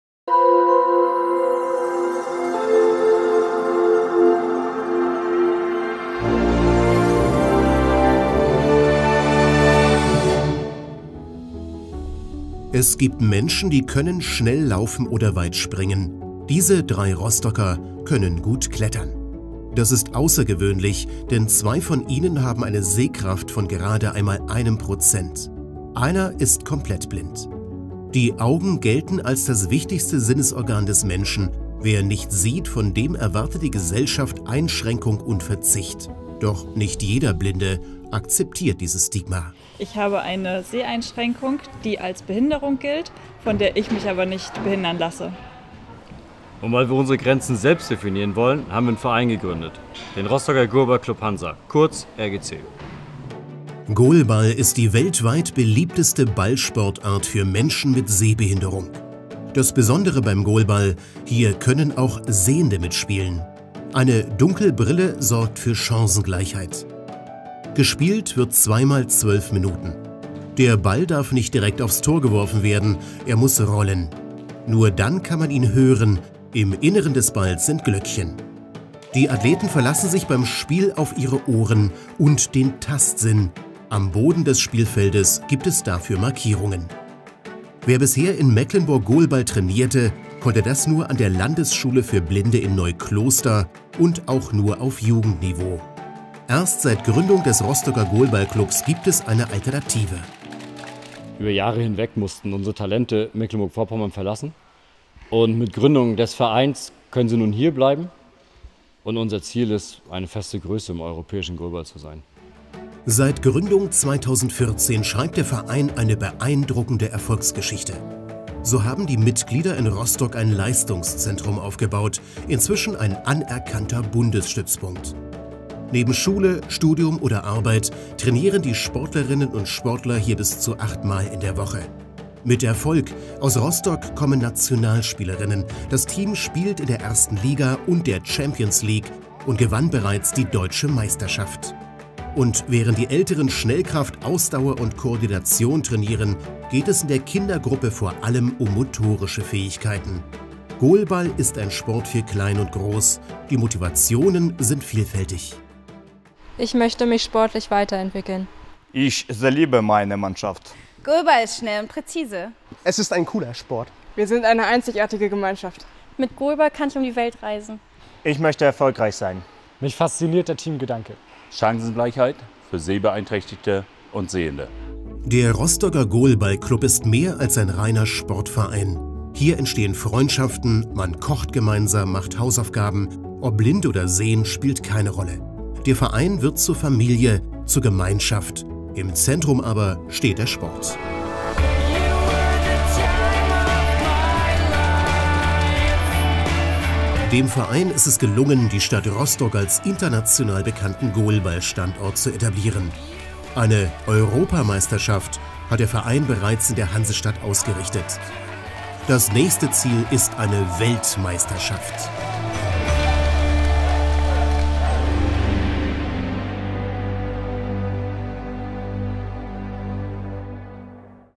Verein des Jahres Mecklenburg-Vorpommern: Rostocker Goalballclub Hansa e.V.
Ein Ballsport, den man blind spielt: Der Rostocker Goalball Club Hansa e.V. weiß, wie das geht. Hier spielen und trainieren Menschen mit und ohne Seheinschränkung gemeinsam und sind auch national erfolgreich. Einzelne Sportler konnten bei den Paralympics Akzente setzen.
Der Verein setzt auf Jugendarbeit und Nachwuchsförderung. Gleichzeitig gelingt es ihm, junge Talente an Mecklenburg-Vorpommern zu binden. Er hat ein treues Publikum. Beim Goalball sind auch die Zuschauer gefordert. Während des Spiels ist es still, denn die Spieler müssen sich auf ihr Gehör verlassen. Ein Glöckchen im Ball soll bei der Orientierung helfen.